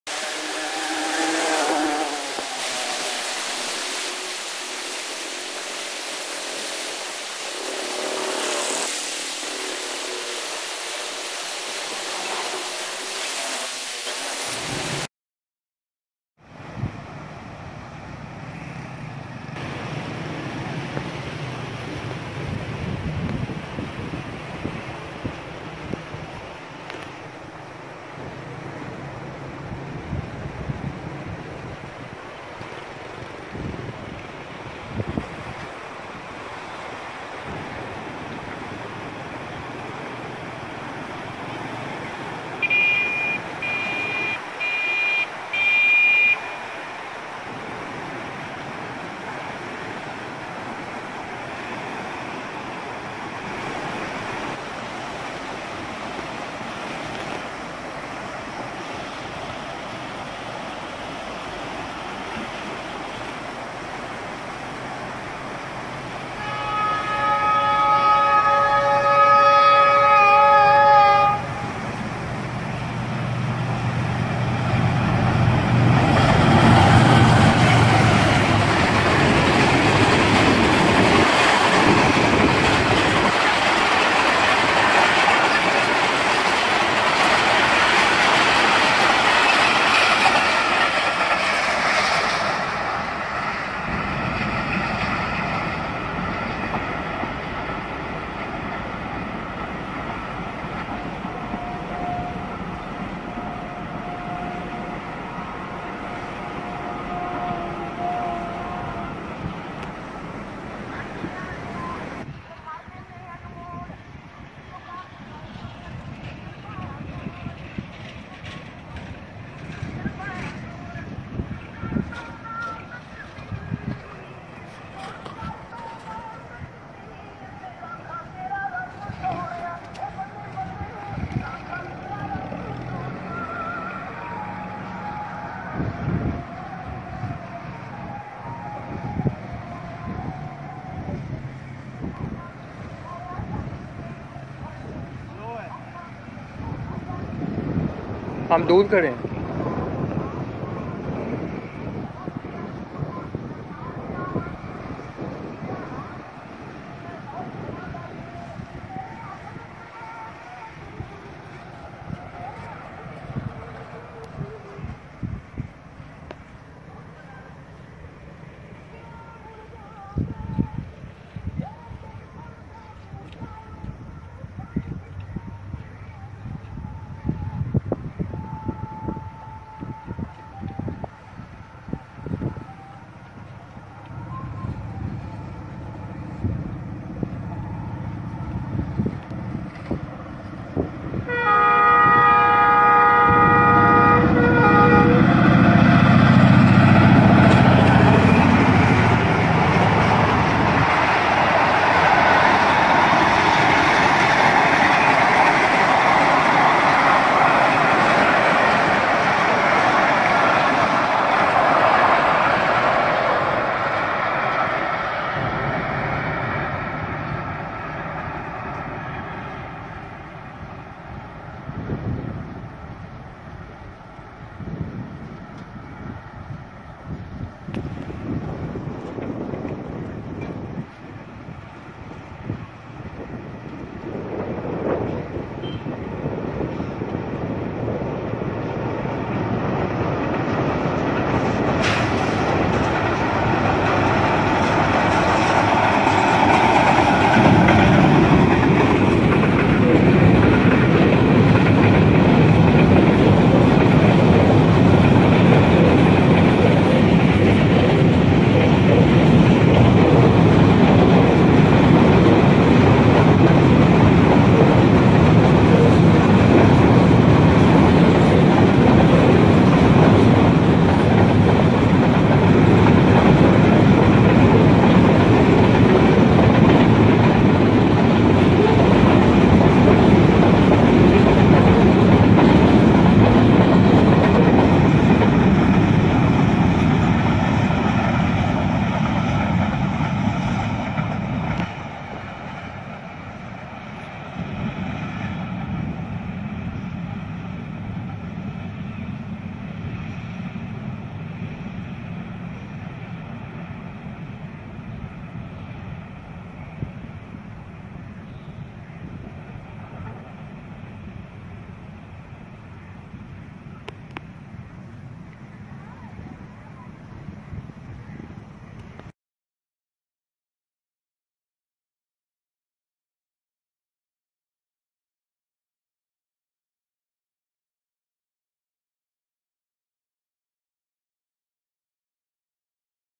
Sensational Arial View Of Express & Container Trains Passing Canal Bridge || Pakistan Railways
Welcome everyone To Next Video On Pakistan Trainz

Pakistan Trainz Present Another Charming View of Trains Passing over Balloki-Sulemanki Canal Bridge Between Pattoki & Changa Manga, Punjab Pakistan
The Lovely Sound of water waves intensify the Beauty. In This Video Include Following Trains.

GEU-40-9055 appear With Rawalpindi Bound Tezgam Express
ZCU-30-6315 Lead Karachi Bound Tezgam Express
GEU-40-9035 Lead Freight Container Train KP

Special Thanks To "Pak Railography" & "Pak Rail Junction" Channels For Support On This Spot.

Details:
Date: ‎21 ‎March, ‎2019
Time: ‎12:16 PM || 3:19 PM || 3:26 PM
Locomotives: GEU-40-9055 ZCU-30-6315 GEU-40-9035
Trains: 7 Up & 8 Dn Tezgam Express & Container Train
Place: Balloki-Sulemanki Canal Bridge, Punjab